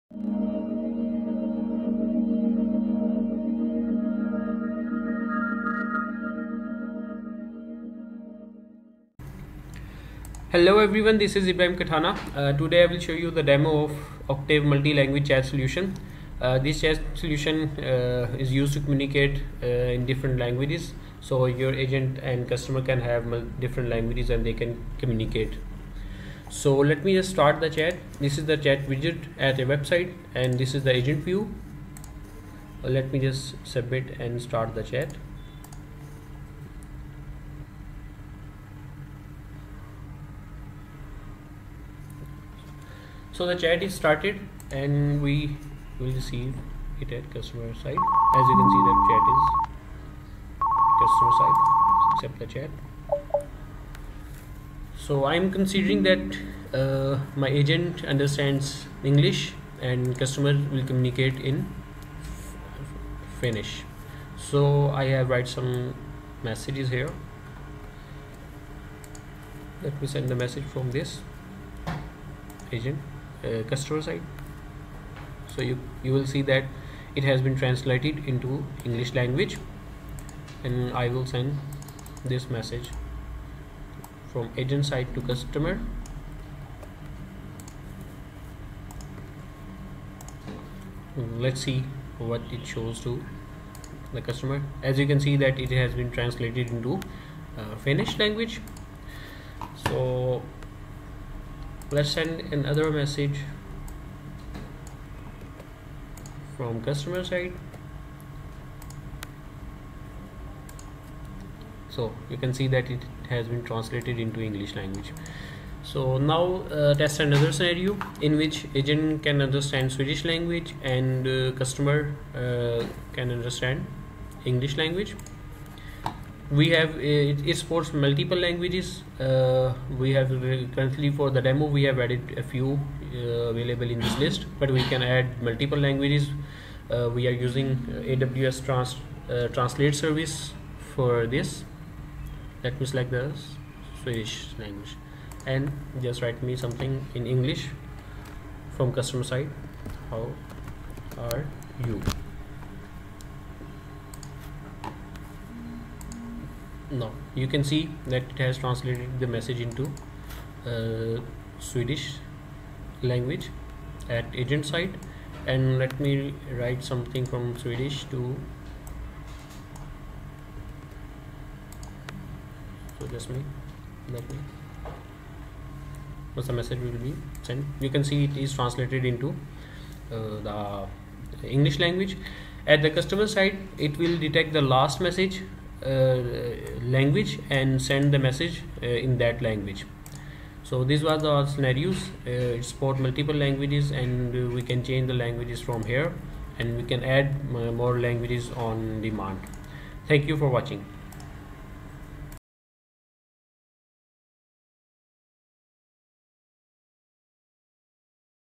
Multi Language Chat Demo for Amazon Connect (Webchat, Facebook, WhatsApp)
Multi Language Chat Demo for Amazon Connect using Amazon Translate.
a customer could message a support agent in their native language, such as French/German/Danish, and the support agent could use their own native language, maybe Italian/English/Finnish, to read and respond to the customer’s messages.
Agent has the ability to change the language in run-time during a chat with the customer.
Solution supports all languages supported by Amazon Connect.

Our Solution support & translates chats from Facebook, WhatsApp, Webchat & other social channels.
Additional languages like Danish, Swedish, Norwegian, Dutch & Finnish are supported.